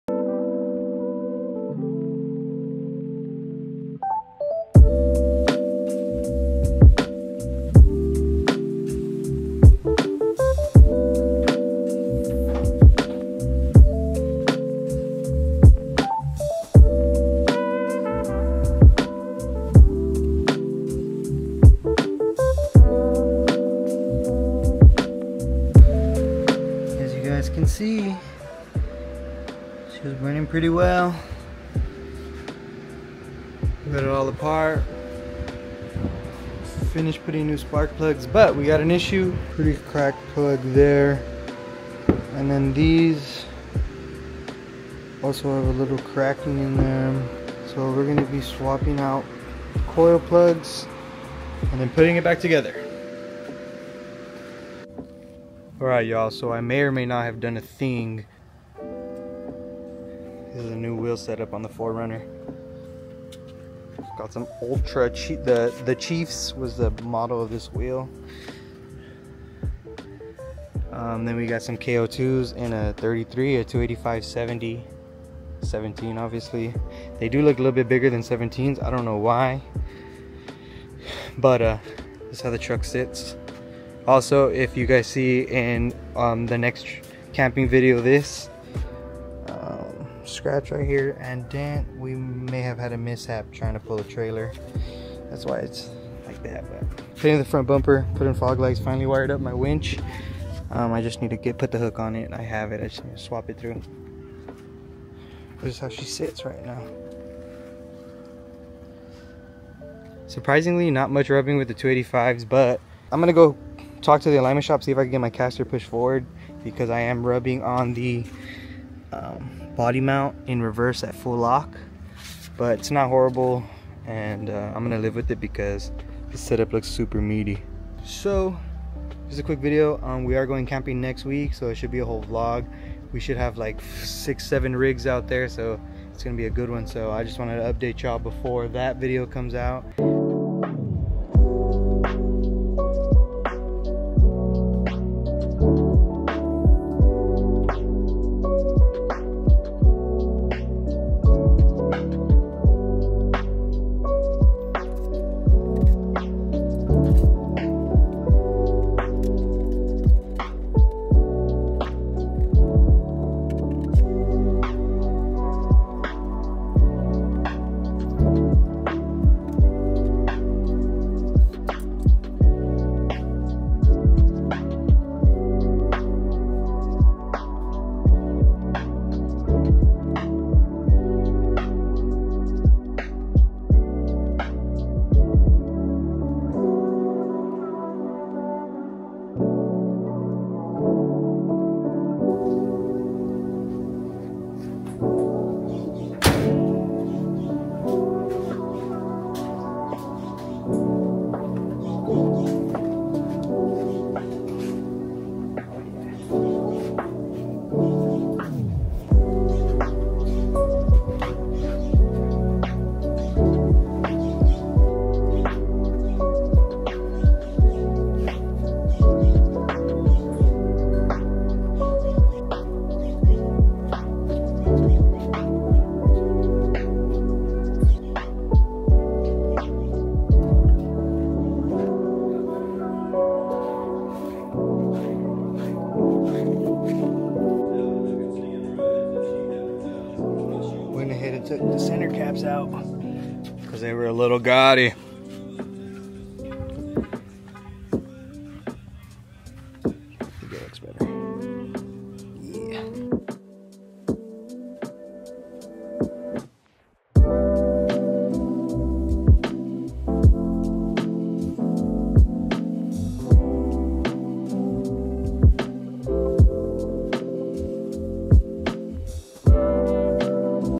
New Mods on my 4th Gen V8 4Runner | 33's FTW!
Maintenance, lights, wheels, and tires. WHAT A DIFFERENCE!

2003 v8 Toyota 4runner sr5 4wd
Bilstein 5100 EMU HD Springs
BAMF front bumper
badlands 12000 winch
lasfit pods / Led's